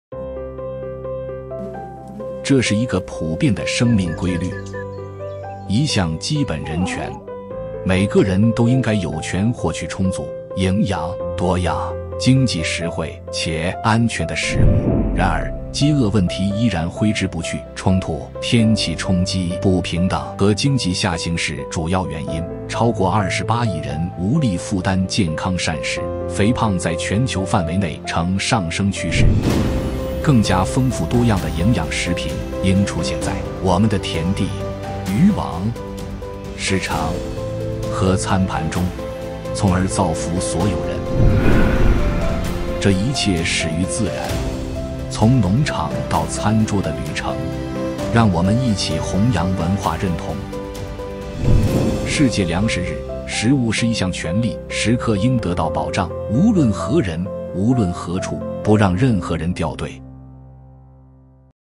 2024年世界粮食日（加长版）
食物是一项普遍的生命法则，也是基本的人权。每个人都应有权获得充足、营养、多样、可负担且安全的食物。然而，饥饿问题依然存在，主要原因在于冲突、天气冲击、不平等和经济下行。

如今，超过28亿人无法负担健康的膳食，而肥胖问题在全球范围内也在不断加剧。为造福所有人，我们需要在田地、渔网、市场和餐盘中提供更加丰富多样的营养食品。

这一切始于自然，是一场从农场到餐桌的旅程，同时也塑造了我们的文化身份认同。世界粮食日提醒我们，食物是一项权利，应该随时得到保障，任何人、任何地点都不应被遗忘。